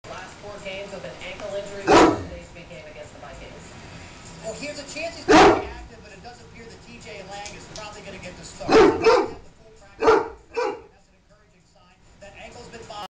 2010 3-6 Great Dane Durango plays daddy to 2 other Danes
Our daddy of the lot of 3 Great Danes, Durango - who is 9 years old, can only take so much of the youngsters playing and he doesn't hesitate to let them know if he has to bring his 185 pounds down there it will be trouble, hehehe. ENJOY